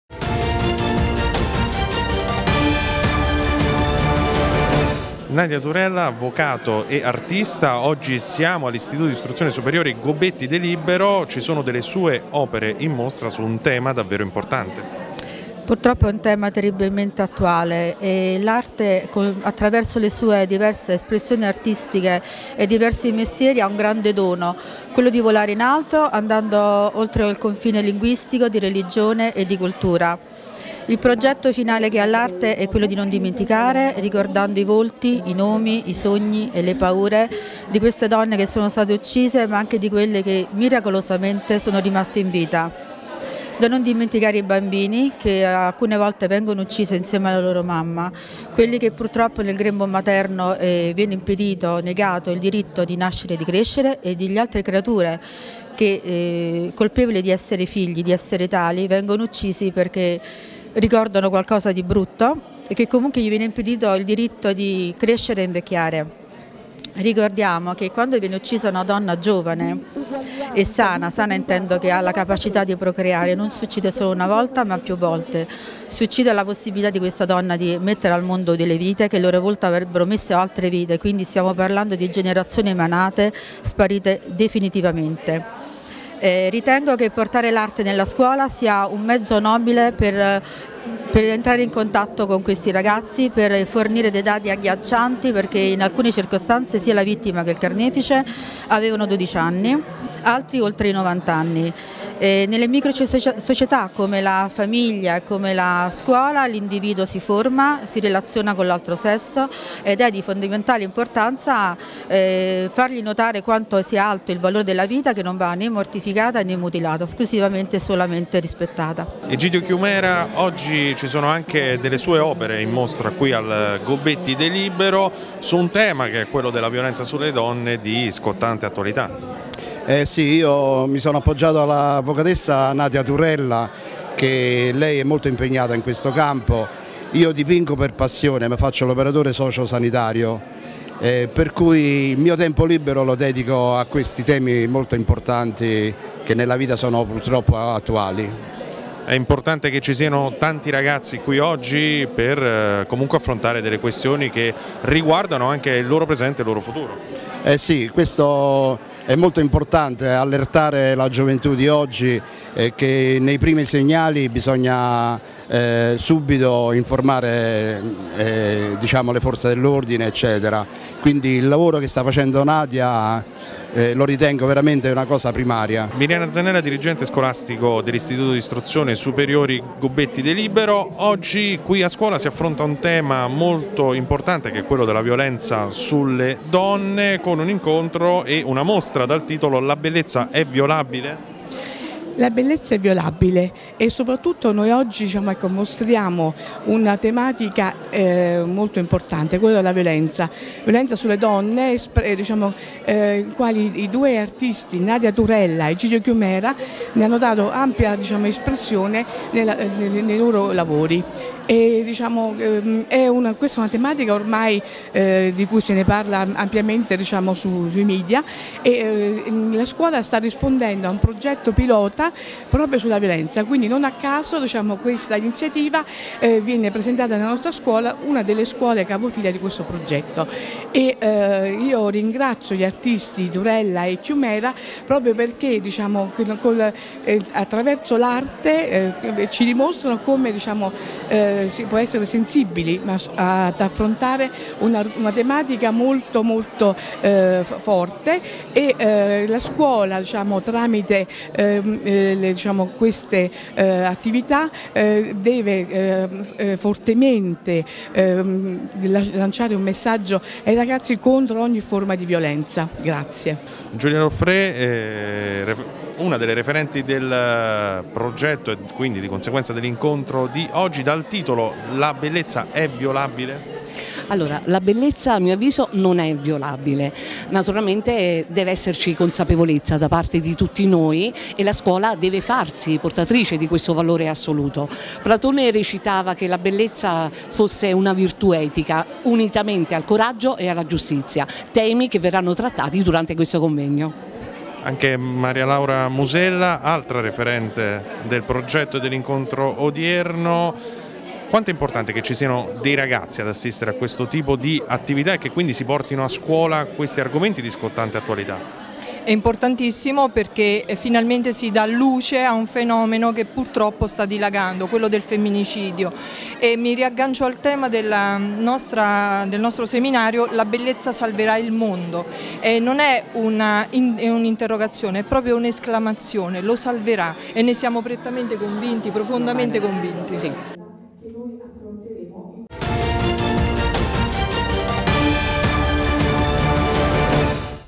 TG7 APPROFONDIMENTO La bellezza è violabile?
L'avvocato e artista Nadia Turella incontra i ragazzi sul tema della violenza sulle donne